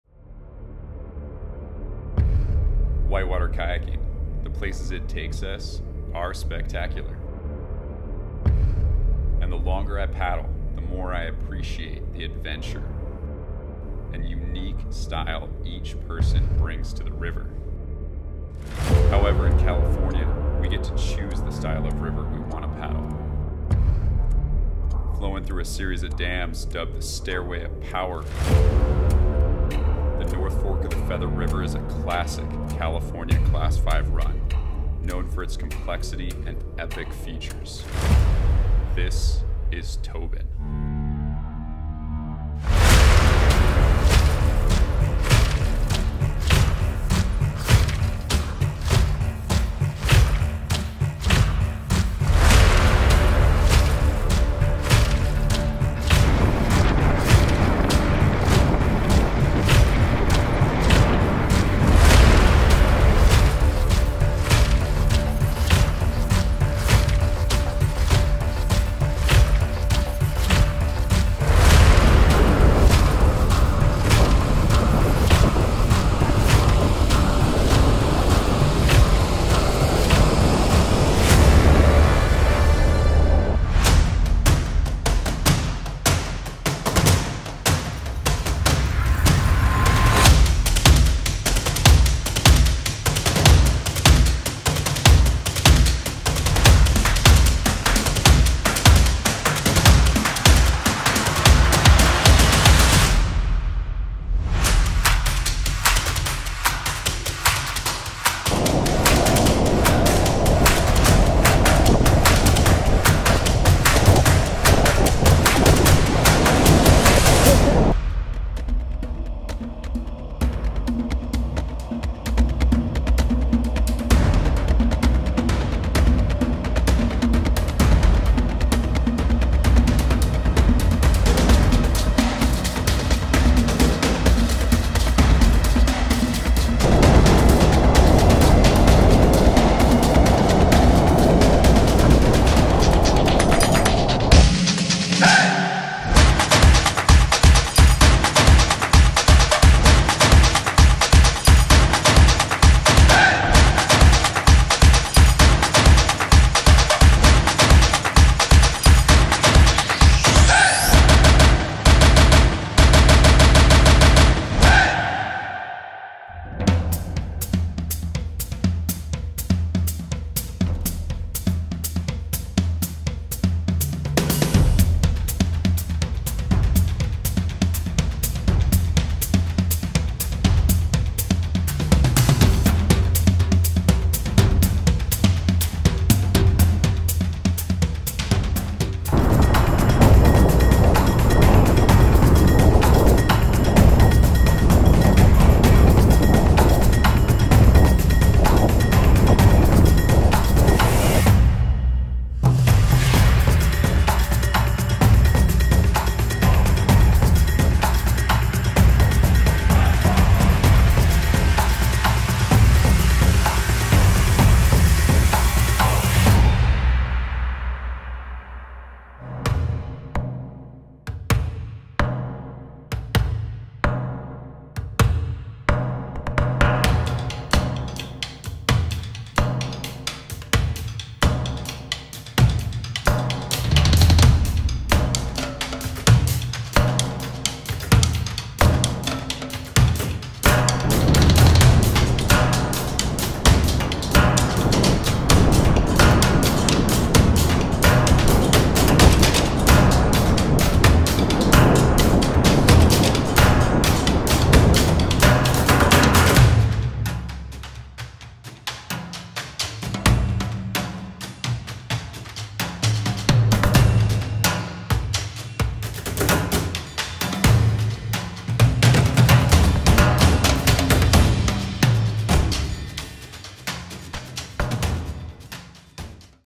Tobin (V) Cinematic Short Film | N.F. Feather River Whitewater Kayaking
1000 CFS. This film brings together the skill of aerial  cinematography with class V whitewater kayaking along the challenging North Fork of the Feather River in Plumas County, CA. The Tobin section is known for its many sieves and hazards that can be difficult to scout from shoreline. Despite this, it remains a booftastic mainstay during its few scheduled releases each year. Which are due to the efforts of American Whitewater and other agencies nonstop vigilance to keep our waterways open for outdoor recreation. Please consider donating to support their efforts 😀

I appreciate all comments and recommendations.

If you are interested in collaborating on a river video please reach out at: